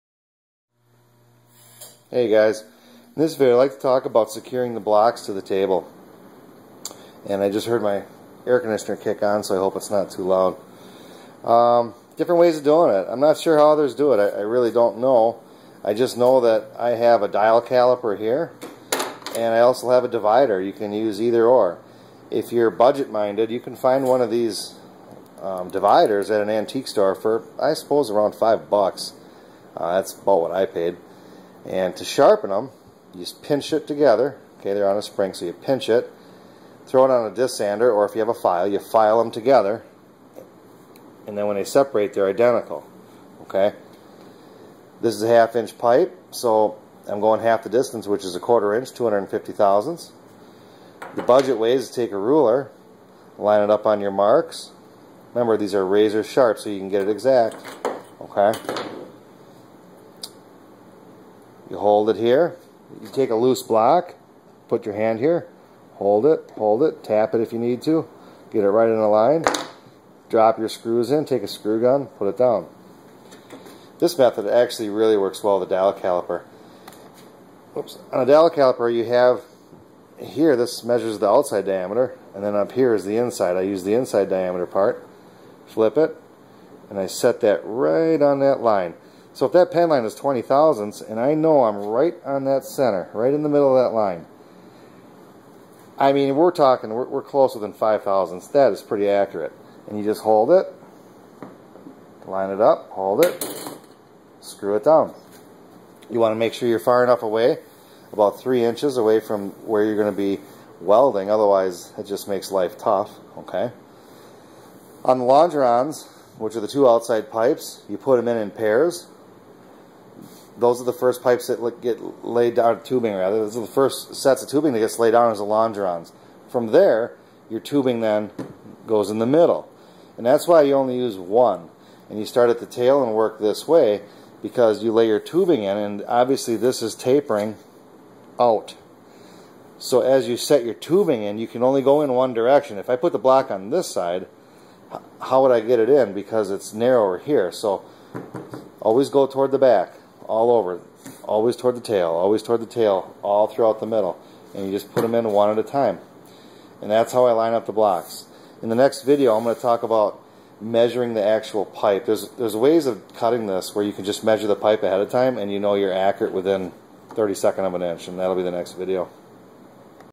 Legal Eagle Ultralight construction video #5
In this video I show my method of screwing the jig blocks down accurately. The Legal Eagle airplane doesn't require many of the blocks.